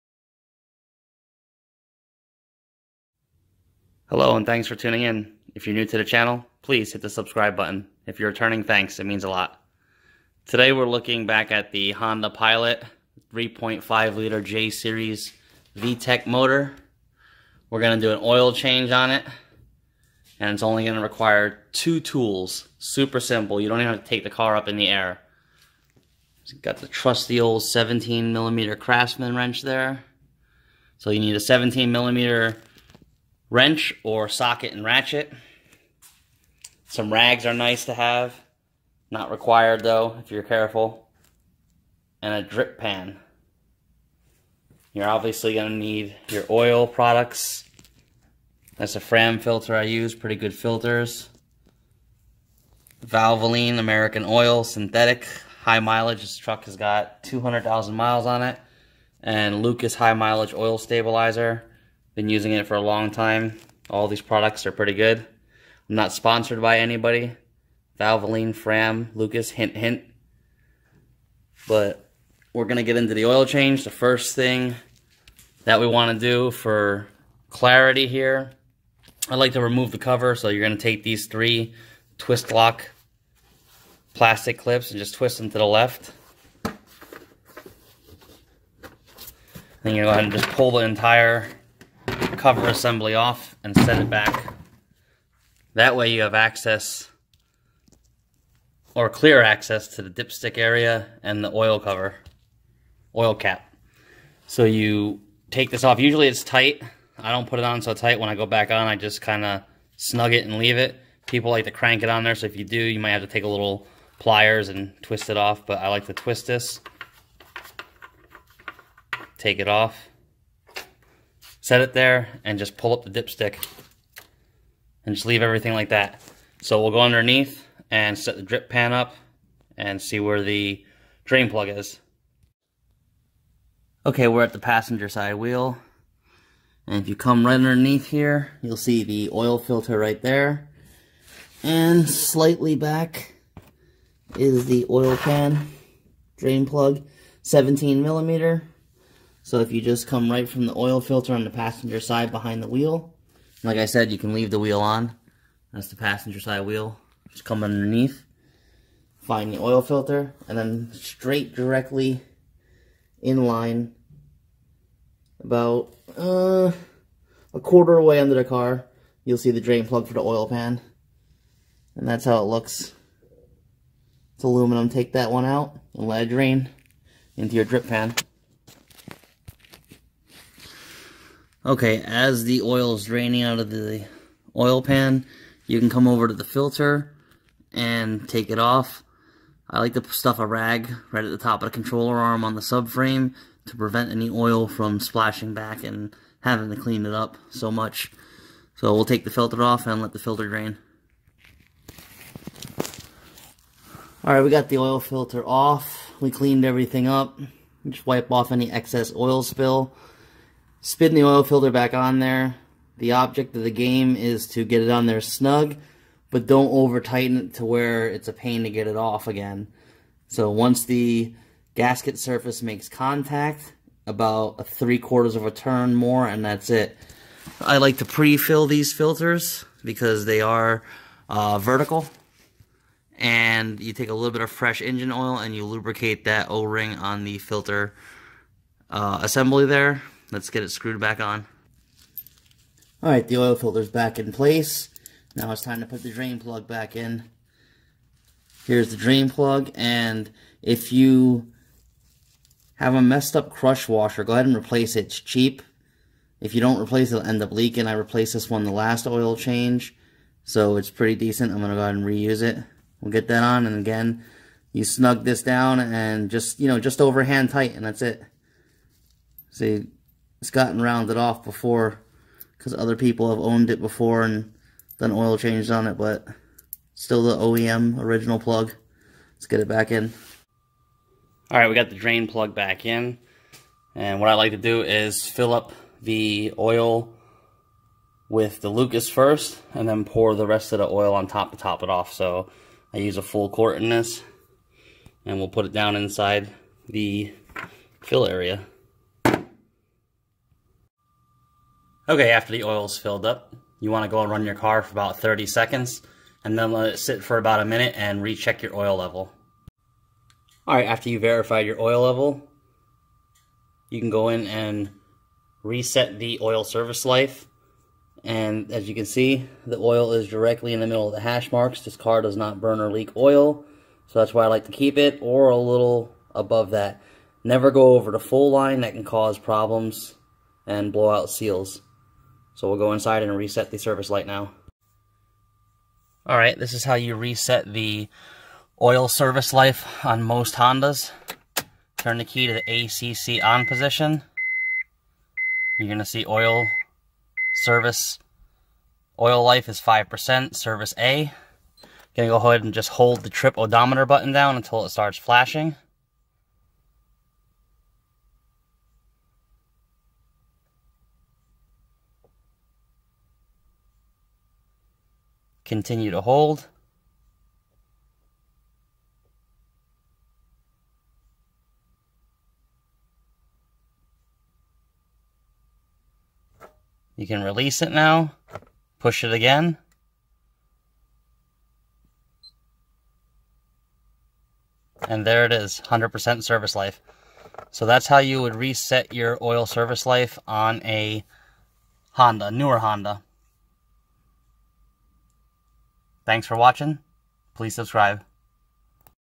Step By Step Honda Pilot/Acura MDX 3.5L Oil Change
Oil changes on a Pilot and Acura MDX 3.5l are very simple and only require the oil, filter, drain pan, and 17mm wrench or socket and ratchet. You don't even have to raise the vehicle. Save yourself some money and  take pride in changing your own oil so you know its done right.